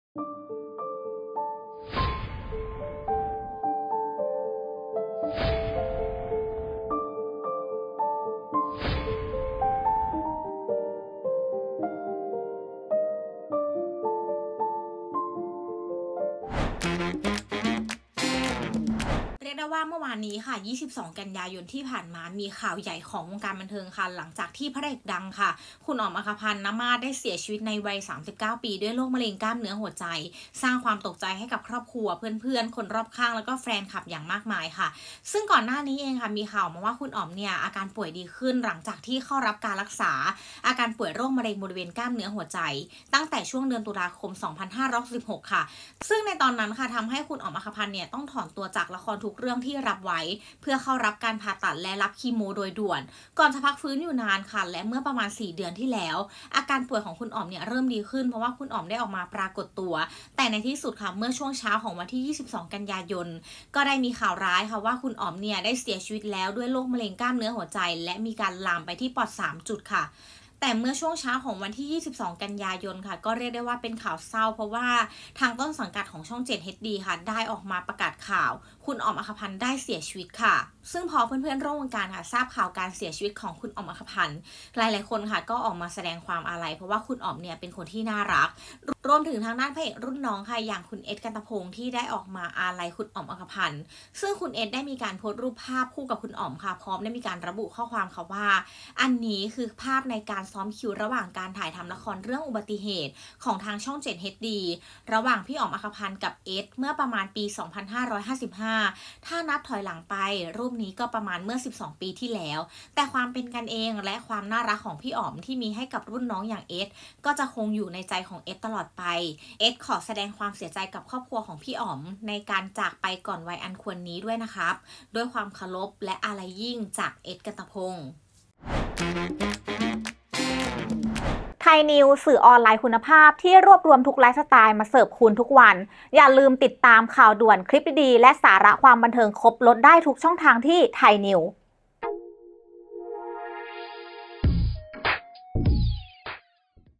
"เอส กันตพงศ์" เผยภาพคู่ "อ๋อม อรรคพันธ์" เมื่อ12ปีที่แล้ว | รู้กันบันเทิง ENT-41-PP
เรียกได้ว่าเป็นข่าวเศร้าข่าวใหญ่ของวงการบันเทิงคสำหรับข่าวการเสียชีวิตของพระเอกดัง "อ๋อม อรรคพันธ์ นะมาตร์" ที่เสียชีวิตในวัย 39 ปี ด้วยโรคมะเร็งกล้ามเนื้อหัวใจ ล่าสุด "เอส กันตพงศ์" เผยภาพคู่ "อ๋อม อรรคพันธ์" เมื่อ12ปีที่แล้ว   ENT-41-PP